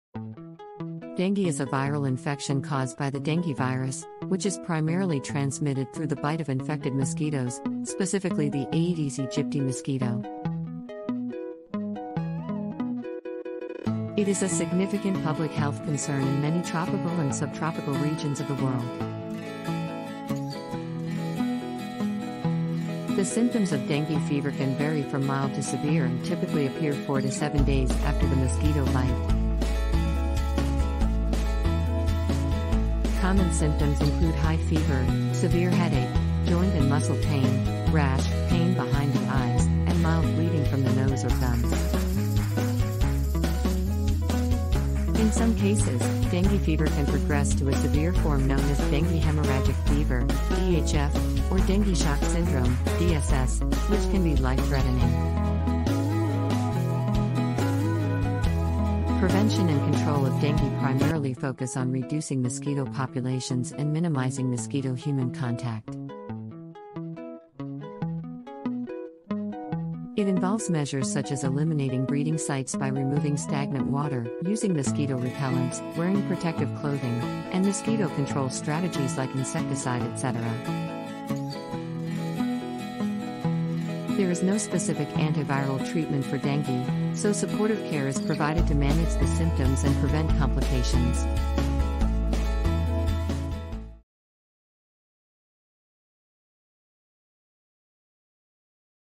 What is Dengue?
Dengue is a viral infection caused by the dengue virus, which is primarily transmitted through the bite of infected mosquitoes, specifically the Aedes aegypti mosquito. It is a significant public health concern in many tropical and subtropical regions of the world.

The symptoms of dengue fever can vary from mild to severe and typically appear 4 to 7 days after the mosquito bite. Common symptoms include high fever, severe headache, joint and muscle pain, rash, pain behind the eyes, and mild bleeding from the nose or gums. In some cases, dengue fever can progress to a severe form known as dengue hemorrhagic fever (DHF) or dengue shock syndrome (DSS), which can be life-threatening.

Prevention and control of dengue primarily focus on reducing mosquito populations and minimizing mosquito-human contact. This involves measures such as eliminating breeding sites by removing stagnant water, using mosquito repellents, wearing protective clothing, and employing mosquito control strategies like insecticide spraying and bed nets. There is no specific antiviral treatment for dengue, so supportive care is provided to manage the symptoms and prevent complications.

It's important to note that dengue cannot be transmitted directly from person to person. Mosquitoes become infected with the dengue virus by biting an infected person and then transmit the virus to other individuals through subsequent bites.

Dengue is a significant global health issue, with an estimated 390 million infections occurring annually worldwide. The disease poses a substantial burden on healthcare systems and can have economic and social impacts in affected areas. Efforts are ongoing to develop vaccines, improve diagnostic tools, and enhance vector control strategies to combat dengue and reduce its impact on public health.